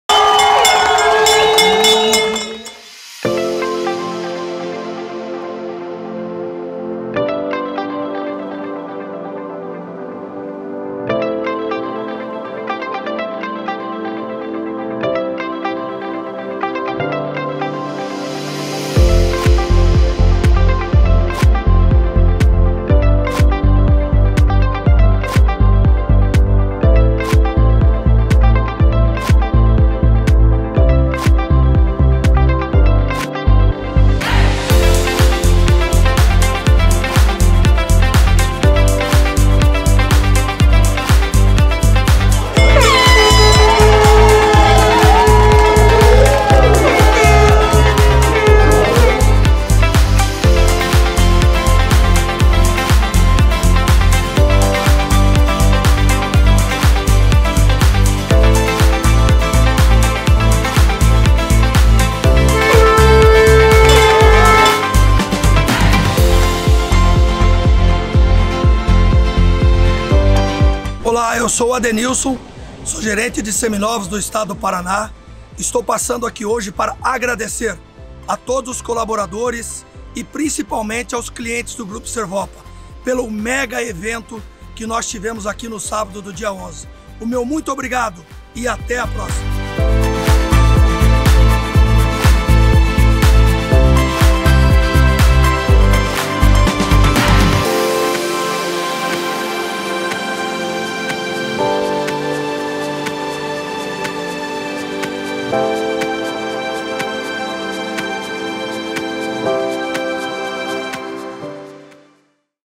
Feirão de Seminovos Grupo Servopa
Vídeo produzido para Seminovos do Grupo Servopa - Feirão de seminovos.

Local: Curitiba - PR